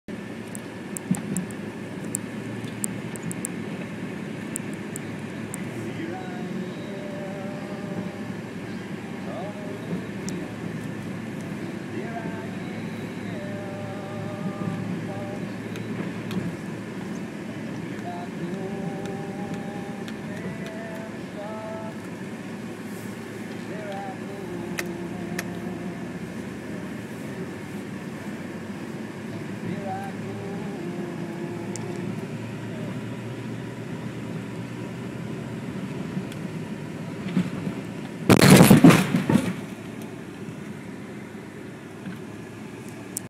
Fisher plow vs Man Hole Cover
Finishing up my plow route when the last manhole of the last street decided to wake me up and pop out 4 of the 8 cutting edge bolts.